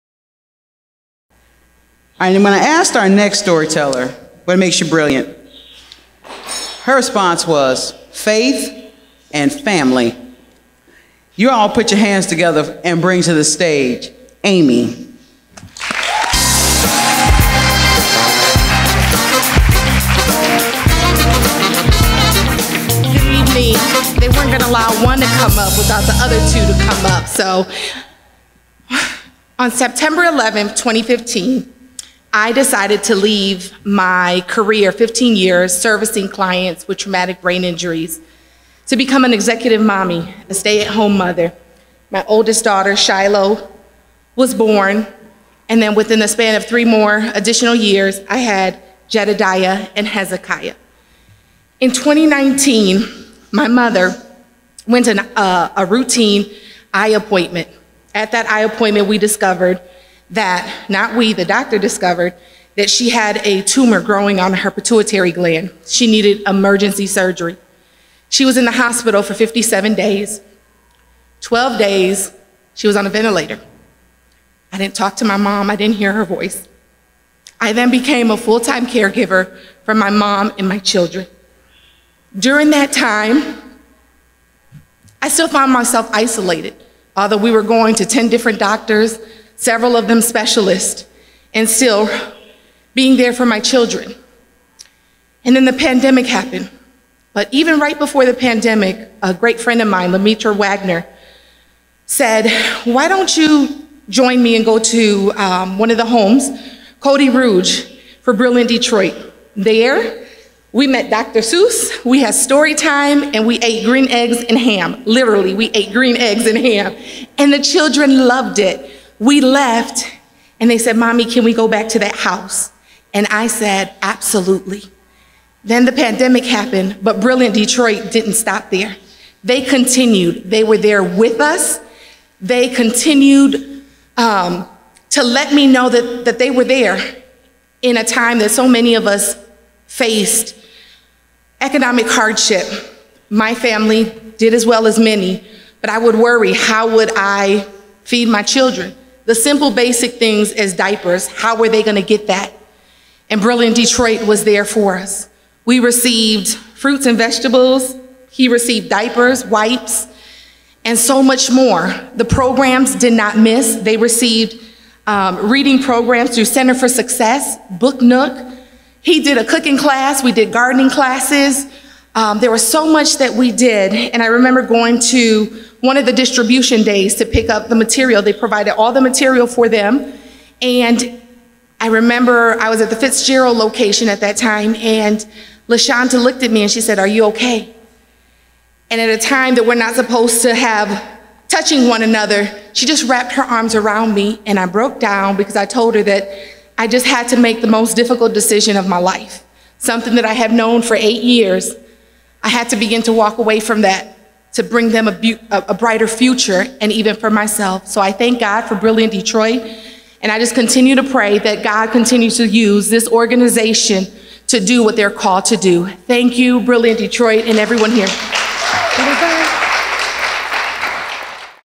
Brilliant Heroes - Amy Bonner
At this year’s Brilliant Detroit annual event, we honored several community heroes. In this video Amy shares how Brilliant supported her during multiple family crises with the help of green eggs and ham. Learn how you can become a part of our Brilliant community + building kid success by visiting